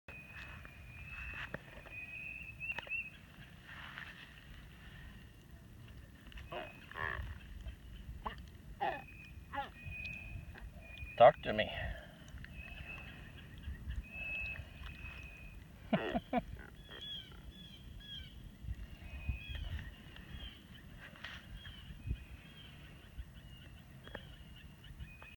Flyrod catfish on articulated Hexagenia nymph
Just a catfish caught on a flyrod